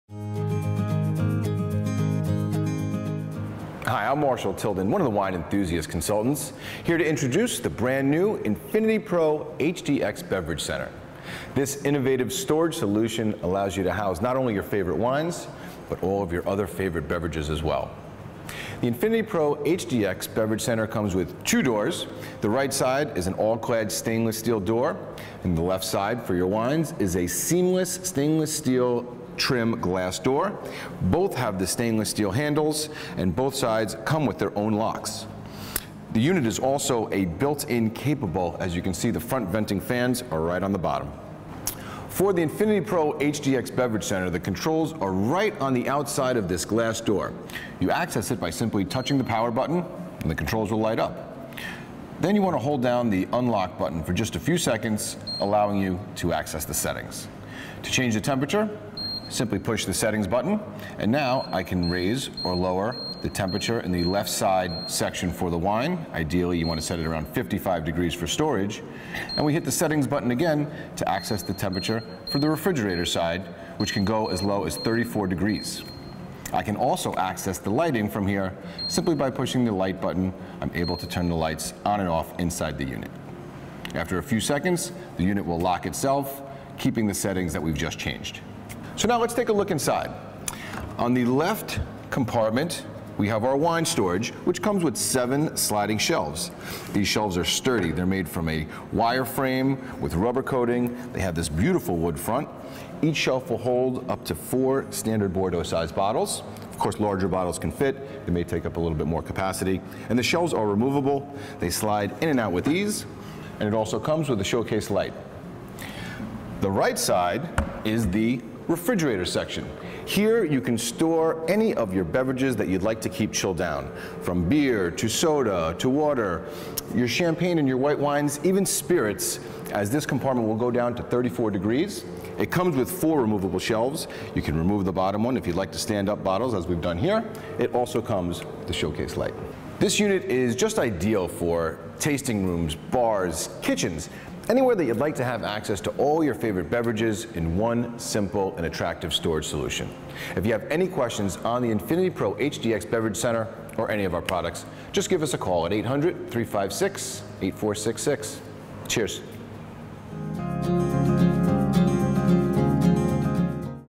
N'FINITY PRO HDX Wine and Beverage Center
N'FINITY PRO Wine Cellars are a hybrid of the most sought-after wine cellar features. All the convenience of higher-end wine cellars at half the price.